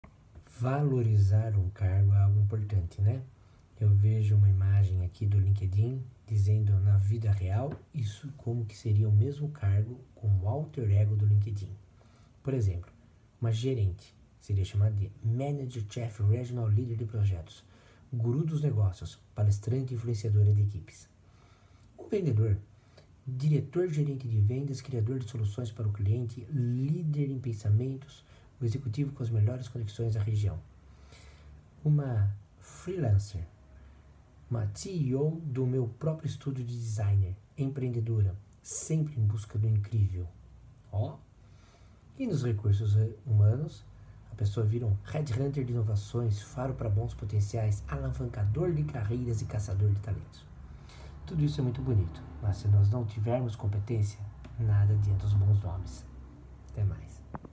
1496. Quando o seu cargo é “maquiado” para mais importante!
EMPREGO -
Os nomes de algumas profissões, quando aparecem no LinkedIn!